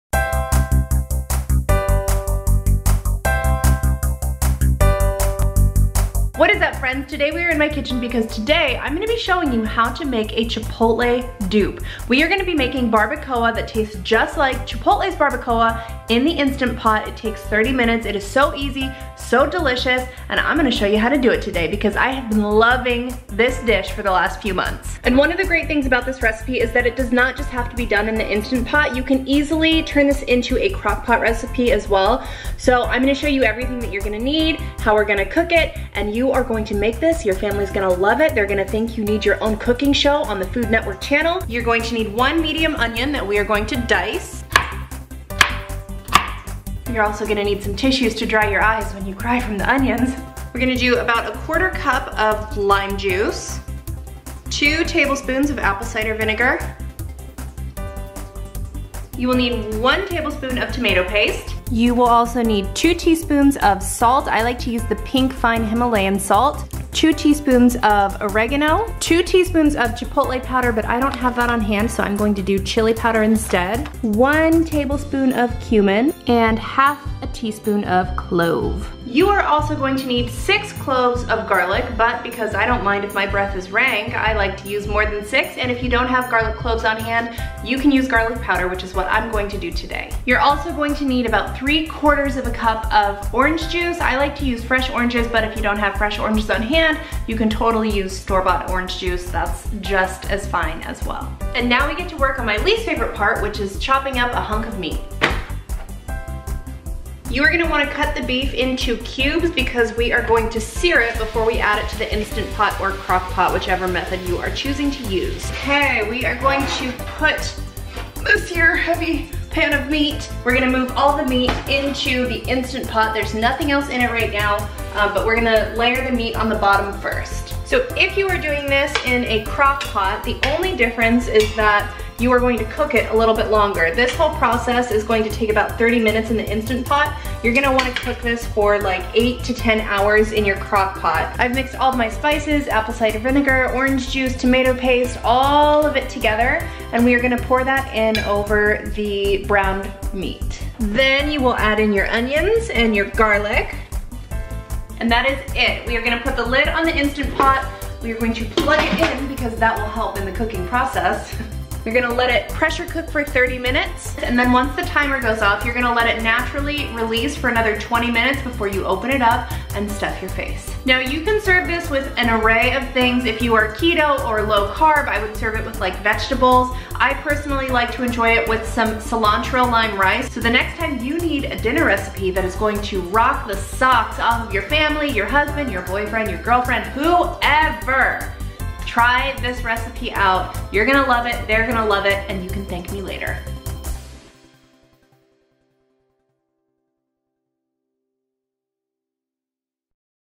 At-Home Chipotle Hack
This 30-minute Chipotle dupe recipe is so fast and easy, and no one will be able to tell the difference! What's your favorite money-saving dinner recipe? 🌯